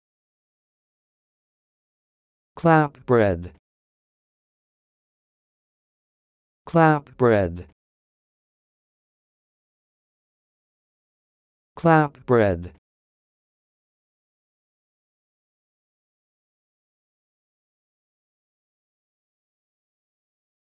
Learn how to say this word: "Clapbread"
This is the definition of "Clapbread":
n.  Alt. of Clapcake